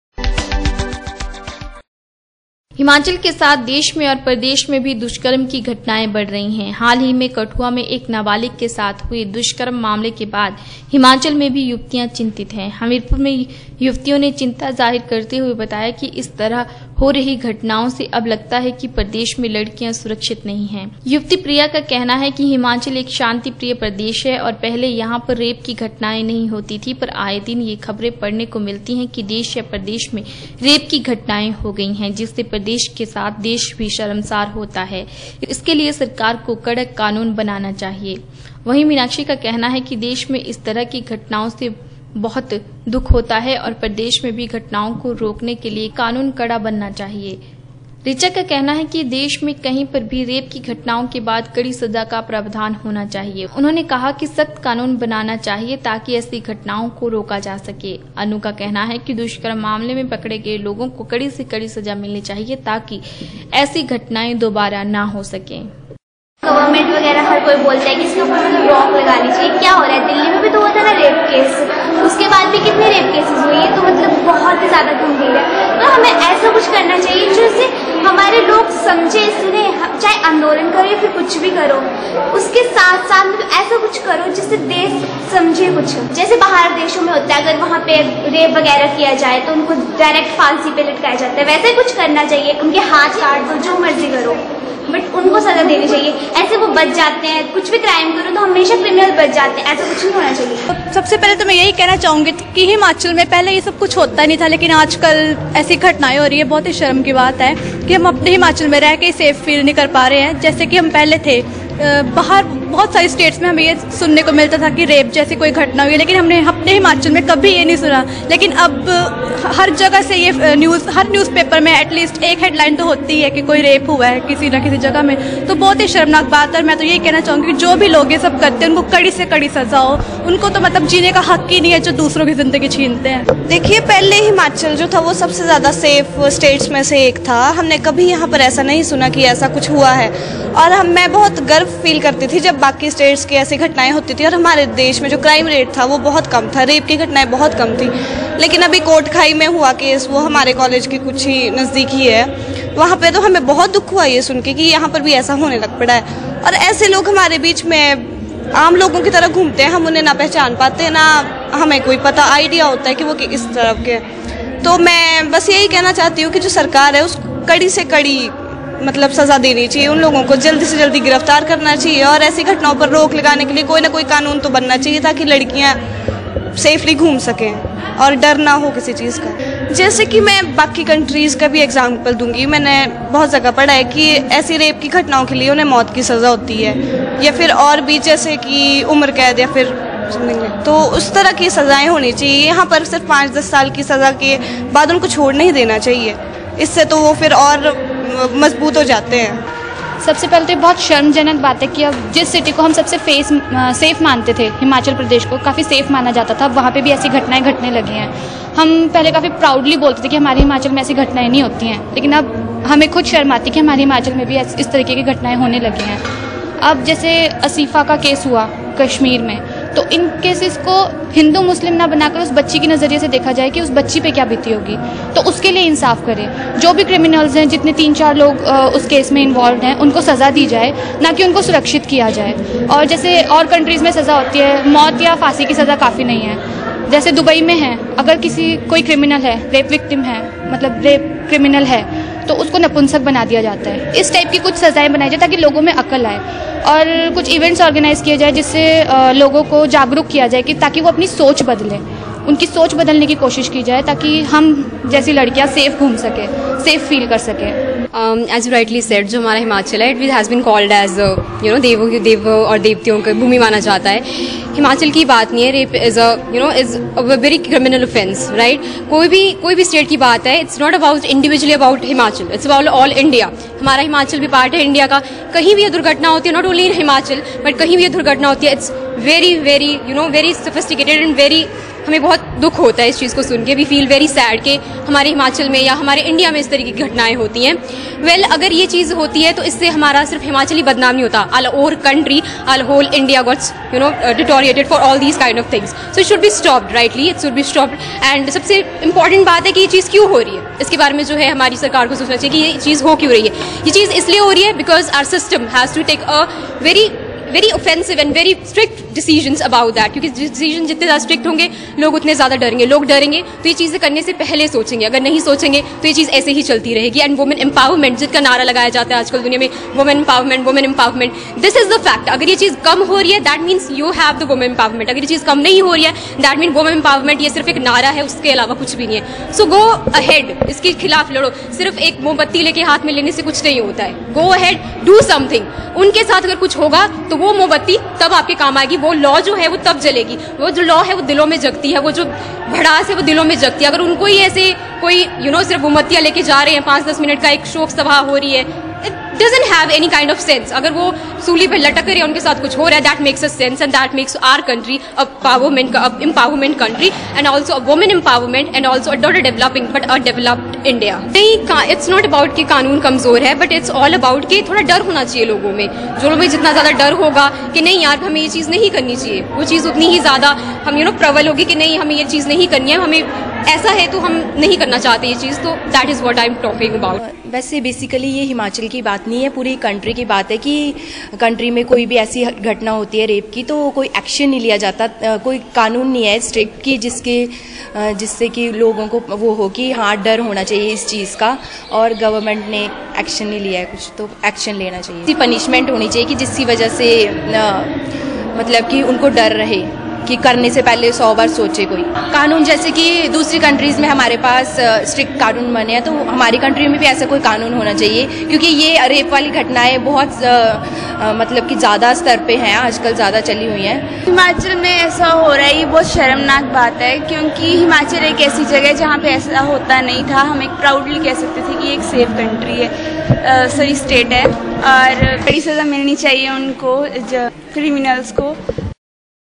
देश में और प्रदेशो में भी दुष्कर्मकी घटनाये बढ़ने के बाद हिमाचल में भी युवतियां चिंतित ।
हिमचाल के साथ देश में और प्रदेशो में भी दुष्कर्मकी घटनाये बढ़ रही है हाल ही में कठुआ में एक नाबालिग के साथ हुए दुष्कर्म  मामले के बाद हिमाचल में भी युवतियां चिंतित है। हमीरपुर में युवतियों ने चिंता  जाहिर करते हुए कहा कि इस तरह हो रही घटनाओं से अब लगता है कि प्रदेश मेंलडकियां सुरक्षित नही ंहै।